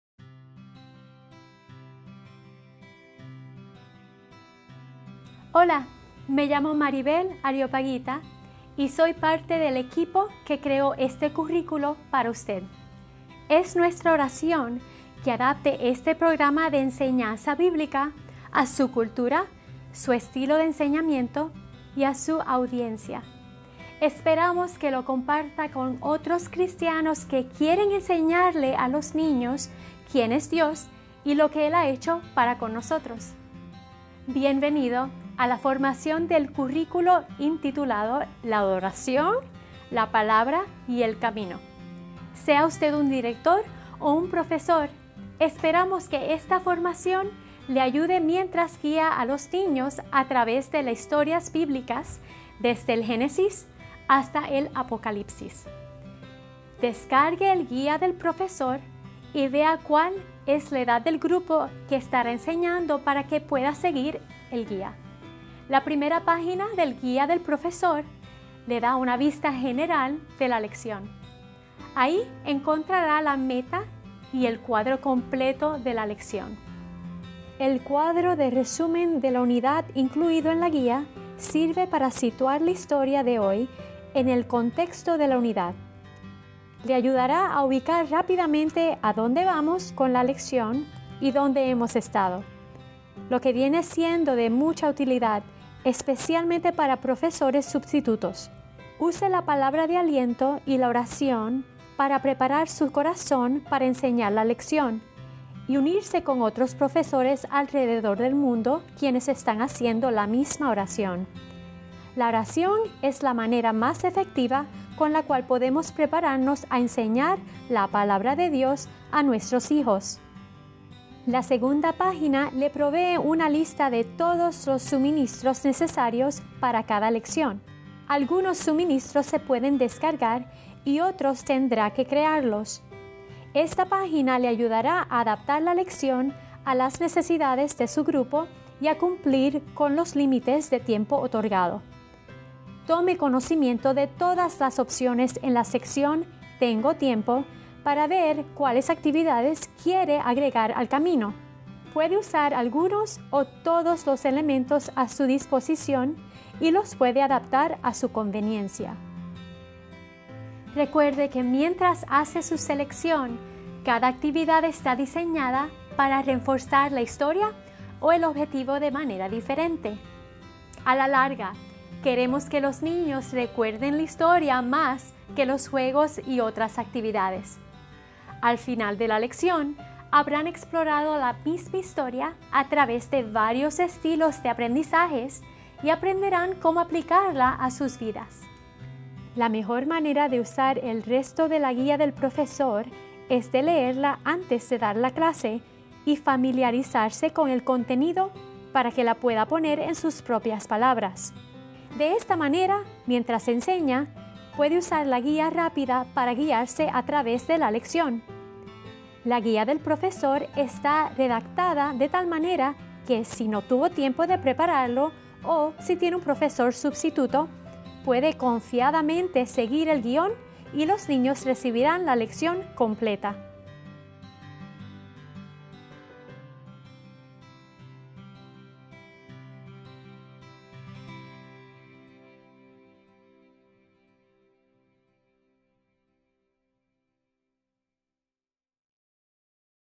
Introduction (Spanish): Worship, Word & Way Teacher Training
Welcome to teacher training for the Worship, Word & Way curriculum (resourcewell.org)! Whether you are a director or a teacher, we hope this training will help you as you walk the children through the Bible stories from Genesis to Revelation. Download a Teacher’s Guide and an At-A-Glance of the age group you will be teaching, so you can follow along.